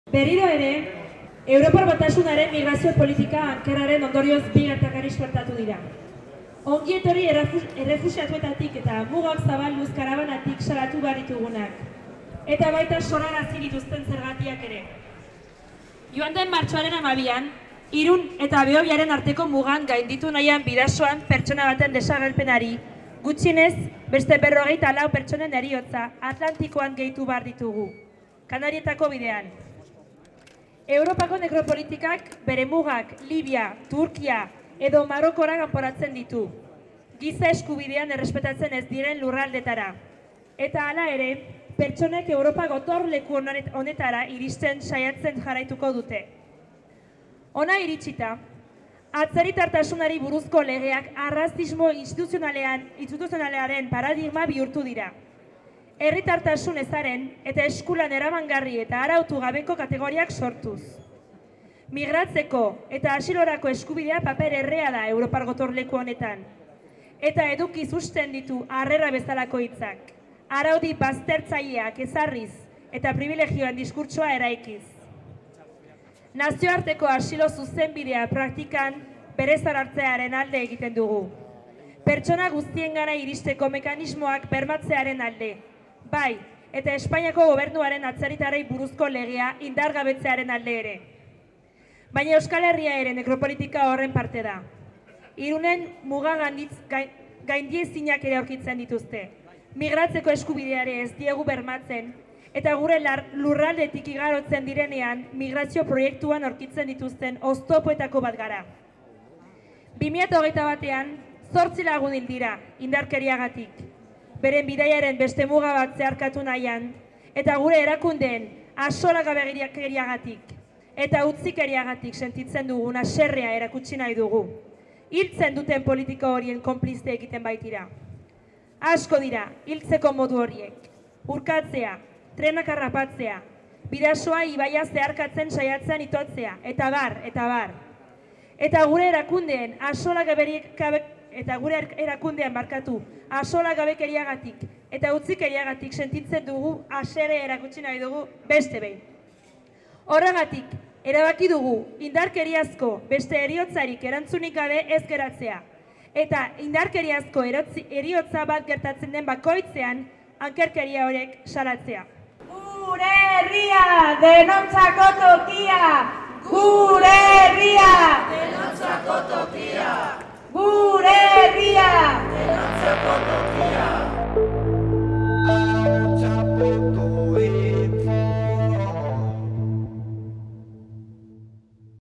"Europa erruduna, Euskal Herria harrera herria" lelopean elkarretaratzea Iruñean
Mugak Zabalduz Karabanak eta Ongi Etorri Errefuxiatuak deitutako elkarretaratzea Iruñeko Udal Plazan, azken egunotan Bidasoa ibaian eta ozeano Atlantikoan hildako migratzaileen memorian.